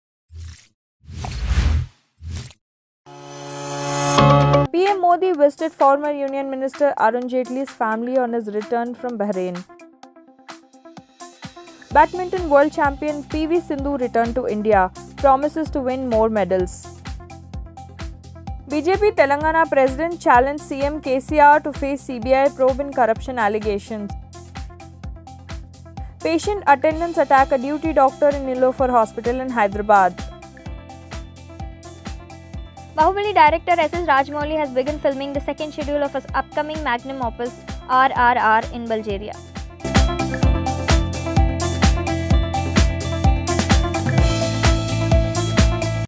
Newsmeter 3:00 PM Bulletin
1. PM Modi visited former Union minister Arun Jaitley's family on his return from Bahrain
2. Badminton world champion P V Sindhu returned to India, promises to win more medals

3.  BJP Telangana president challenged CM KCR to face CBI probe in corruption allegations.

4. Patient attendants attack a duty doctor in Niloufer Hospital in Hyderabad.

5. Bahubali director S.S. Rajamouli has begun filming the 2nd schedule of his upcoming magnum opus "RRR" in Bulgaria